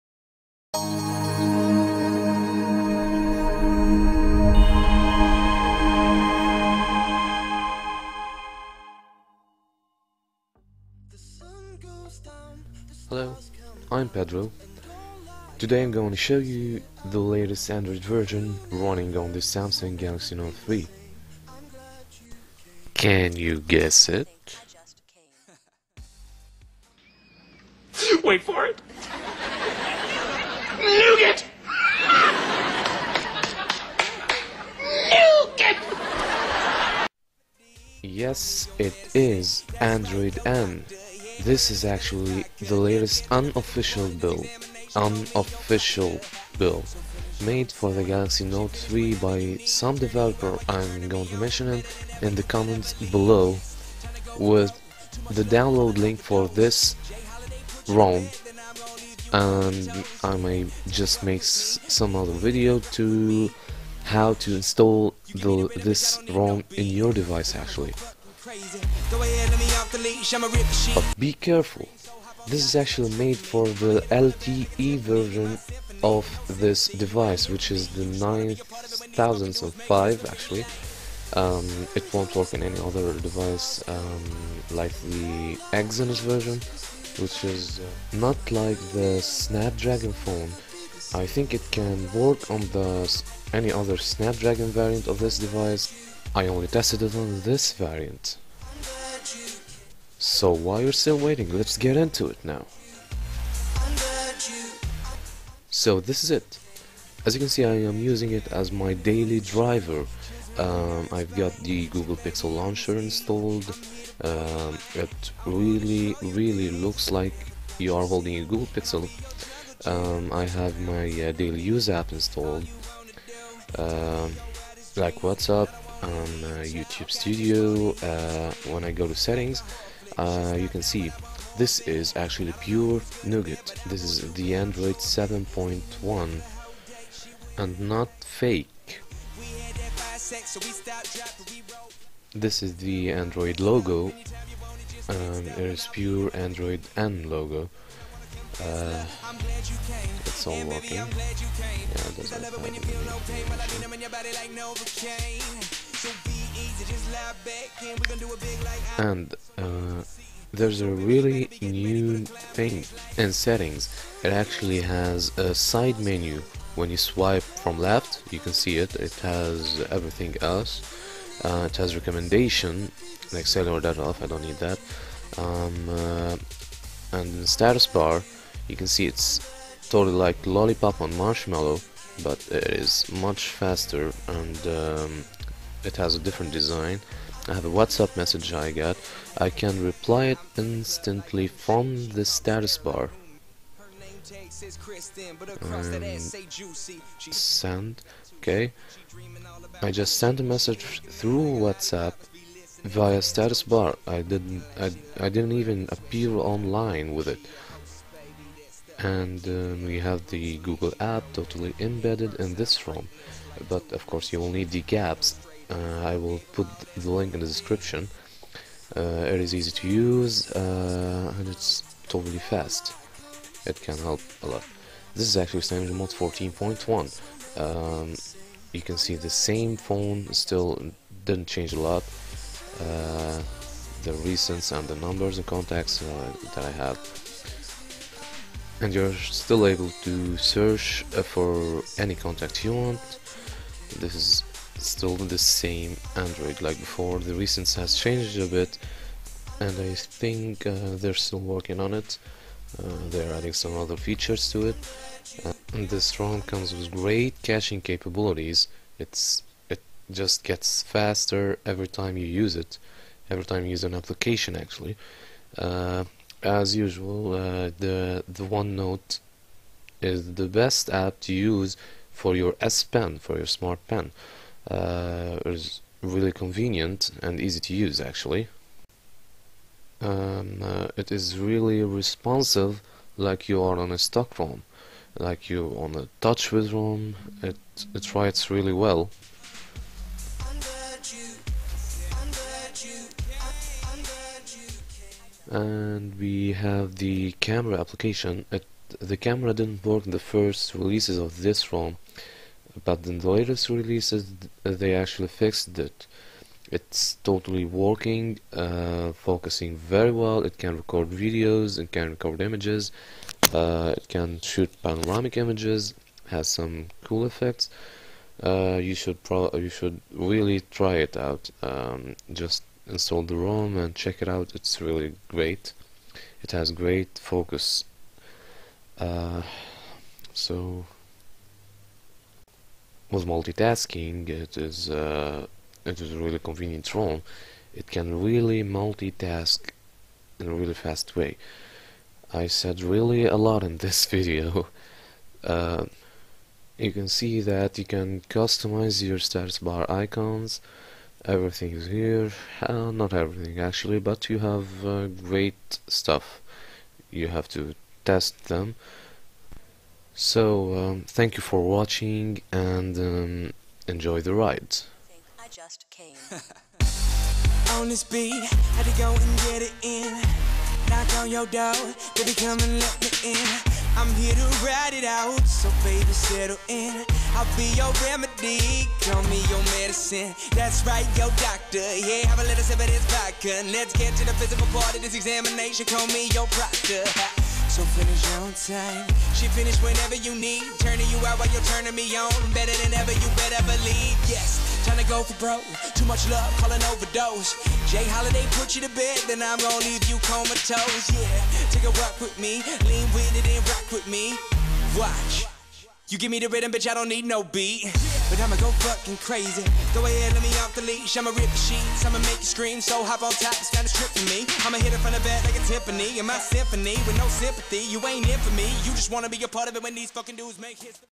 CM14.1/Android 7.1 Galaxy Note 3 LTE N9005 Latest Update
This is a preview of the Android N(7.1) on the Samsung Galaxy Note 3
ROM Developer: trader418